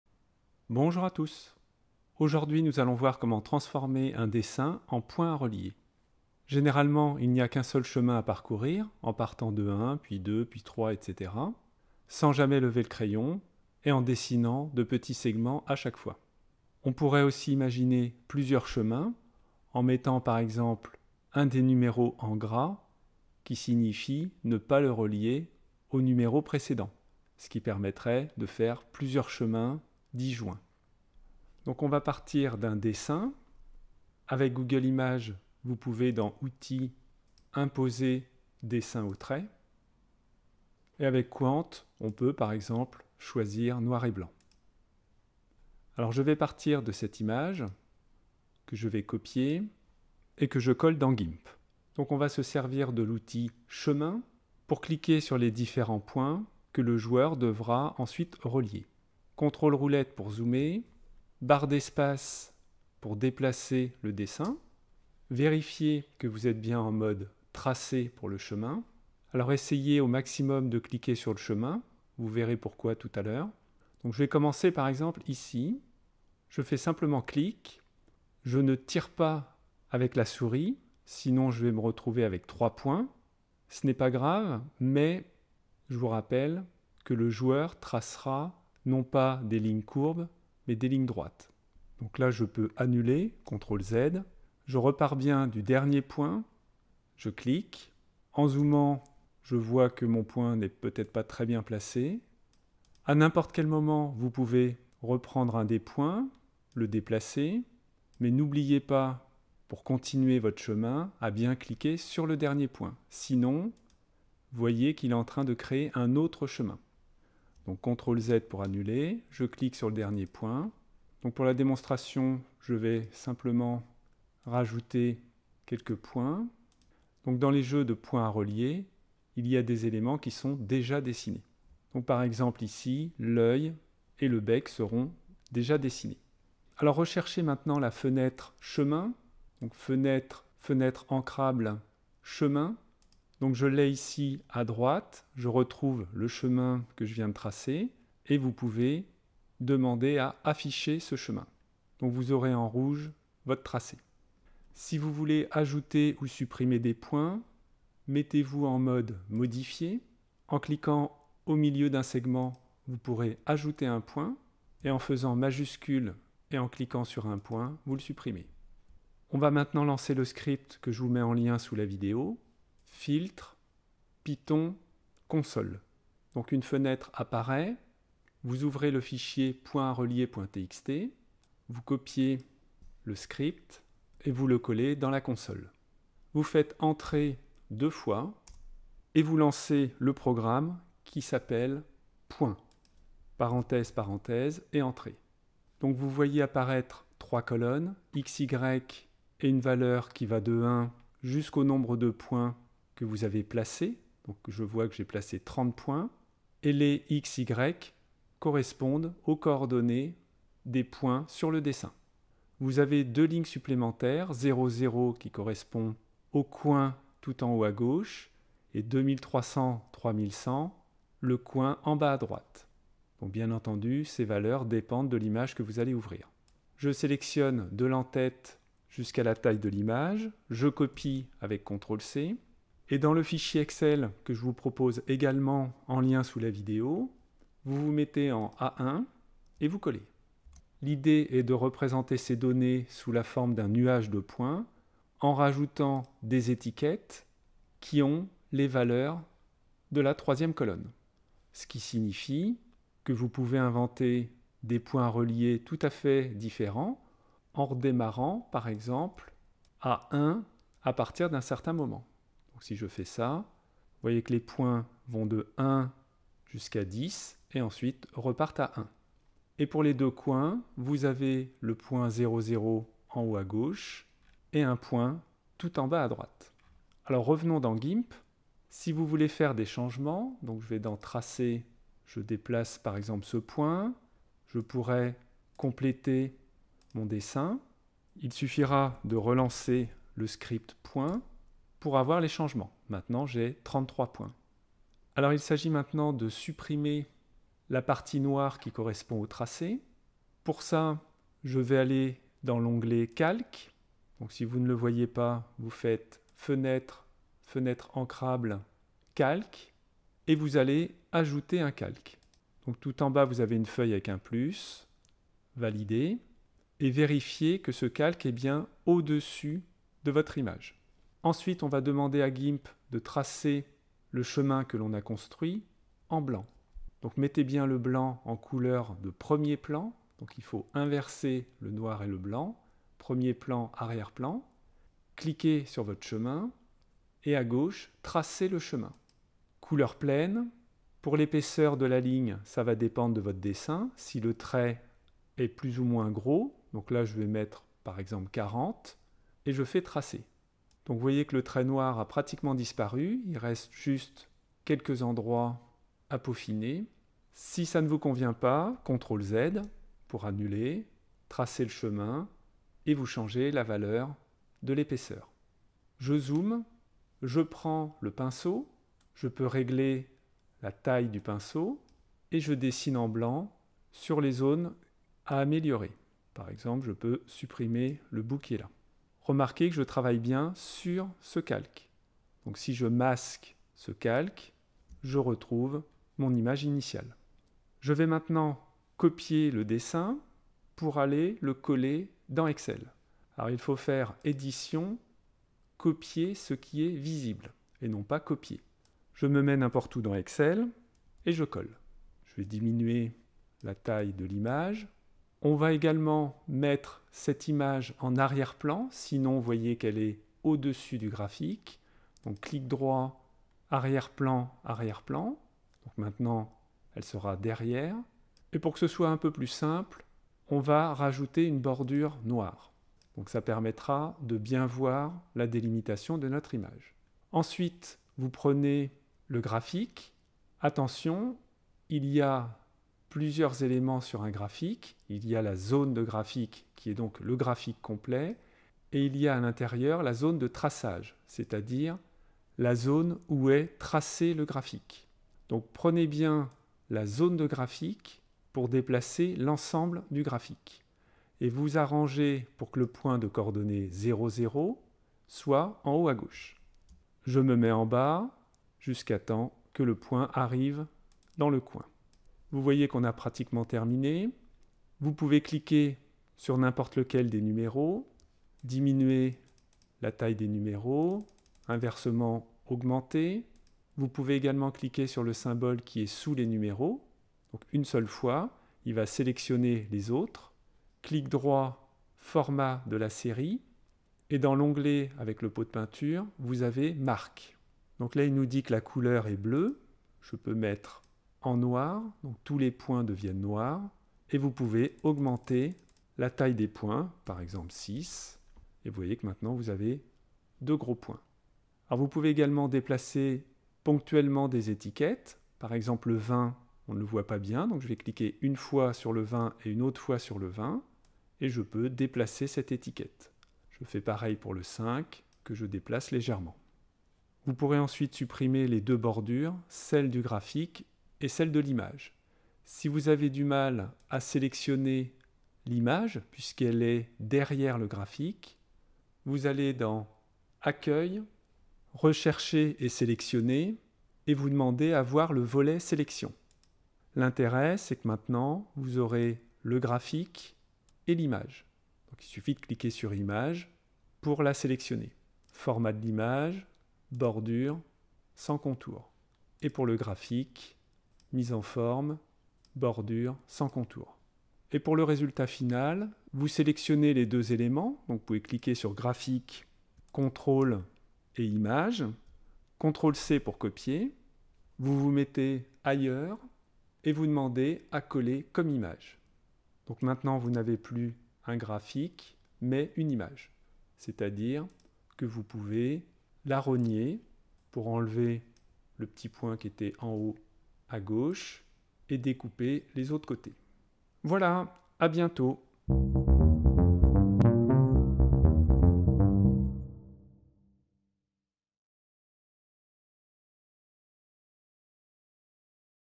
Excel + Gimp : Transformer une image en points à relier
Voici une vidéo expliquant comment transformer un dessin en points à relier en utilisant Gimp et Excel.